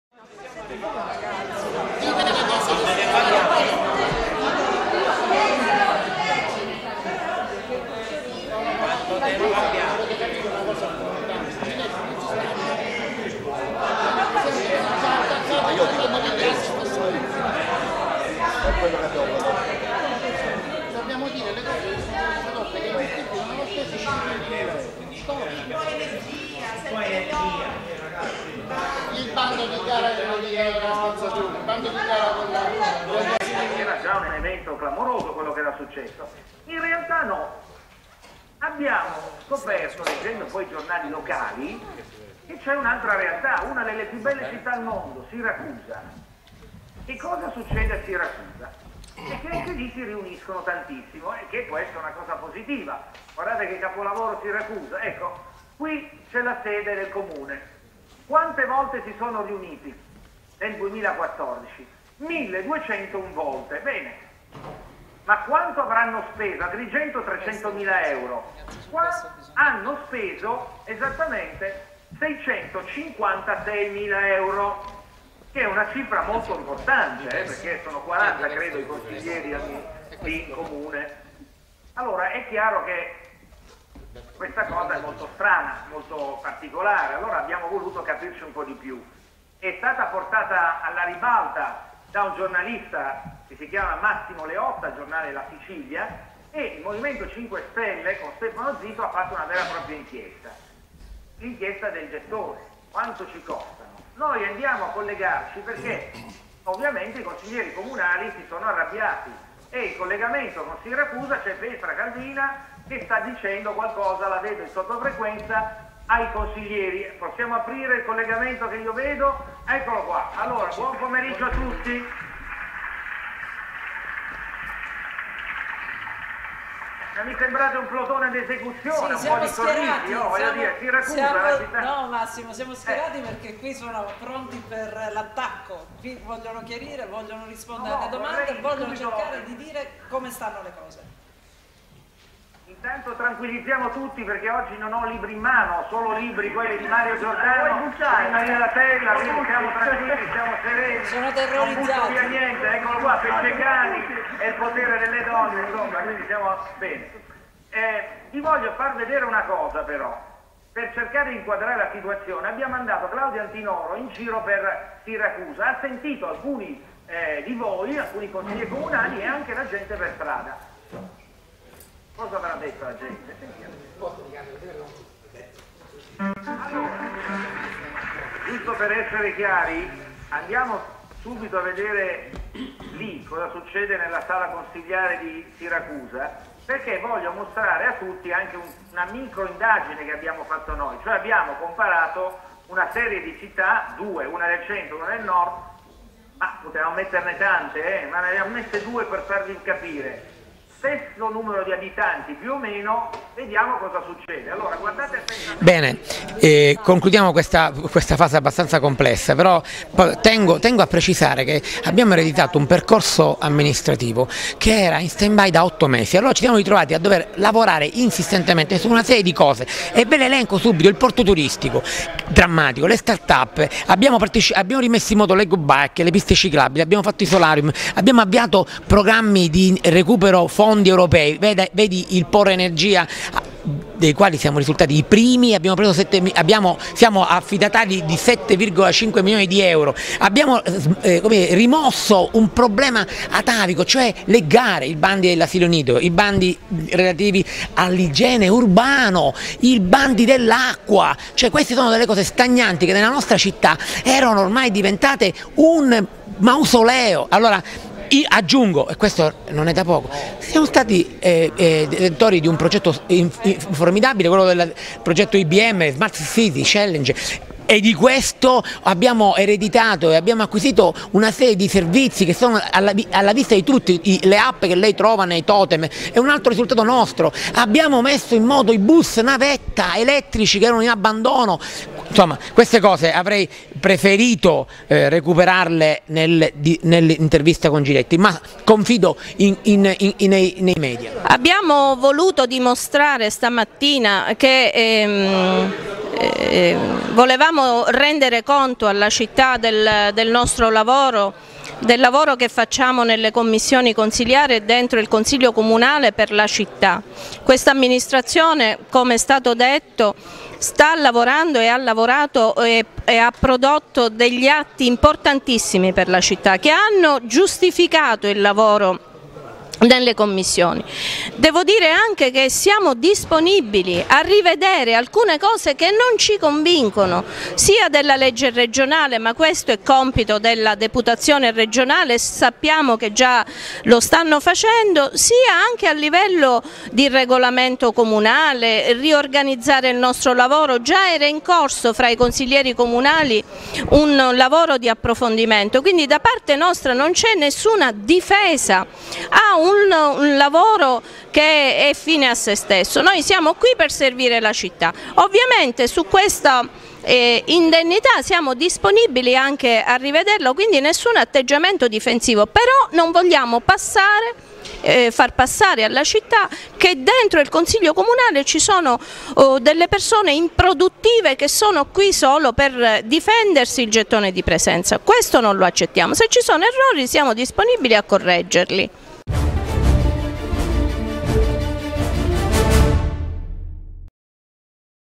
SIRACUSA AL CENTRO DELL'ARENA NAZIONALE
Al quarto piano va in onda il massacro della città - Le scene più vere e crude sono state quelle del backstage e dei fuori onda - Il commento dei consiglieri Francesco Pappalardo e Carmen Castelluccio